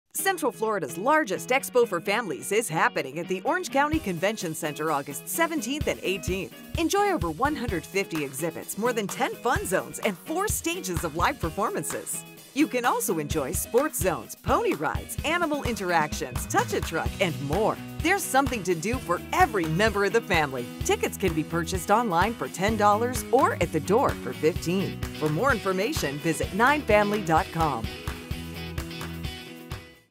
Florida Kids and Family Expo WFTV Commercial
@wftv is a proud partner of the Florida Kids and Family Expo. Thank you, WFTV for the commercial you will be airing and sharing this year! We're so grateful to have you included in this year's expo again! Get all the details on this epic expo